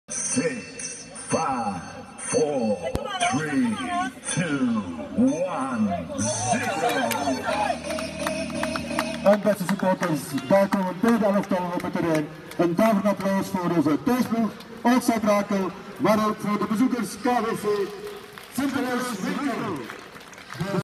U8's Olsa Brakel begeleiden spelers eerste elftal
voor de aanvang van de wedstrijd tegen leider Sint-Eloois-Winkel